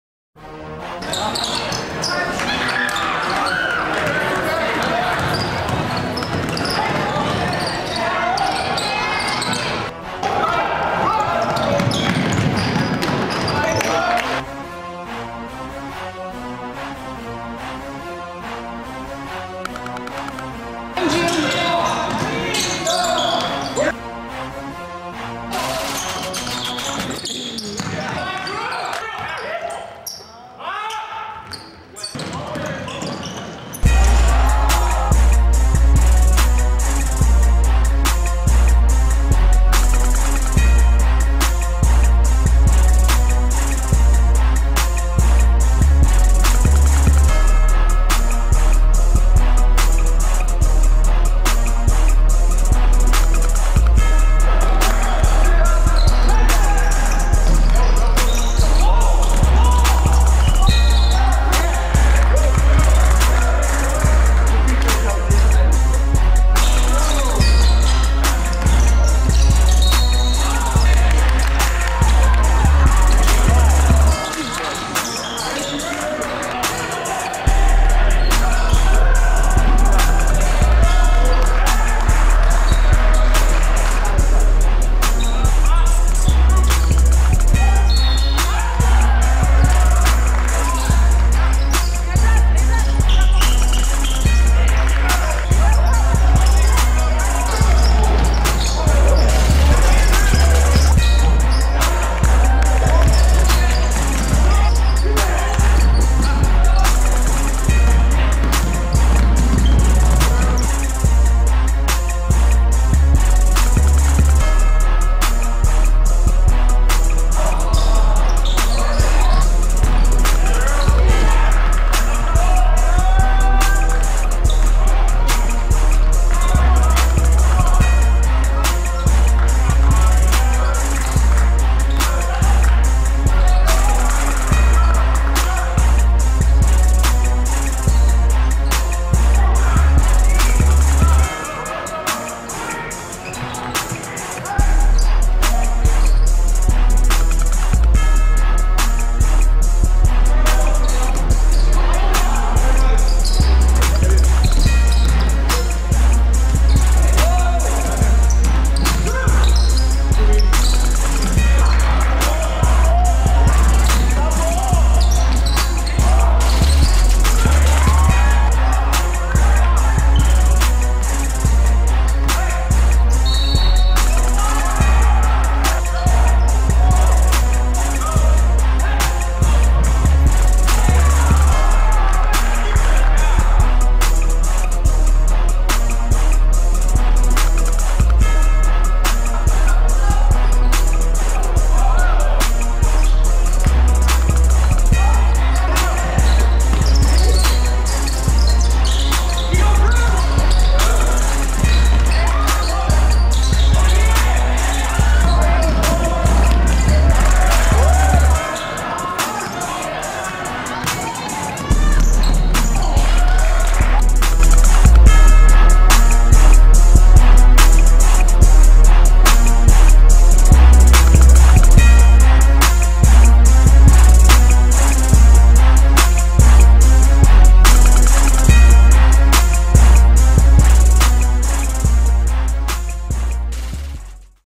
Andrew Robinson #3
6'3 Athletic Senior Point Guard From NYC. Goes to Pomfret School. NEPSAC